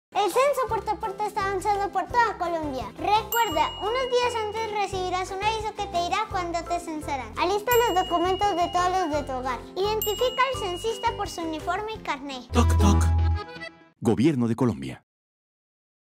Censo puerta a puerta avanza por toda Colombia
Los colombianos seguimos conectados con el censo, deja pasar al censista y atiende la visita.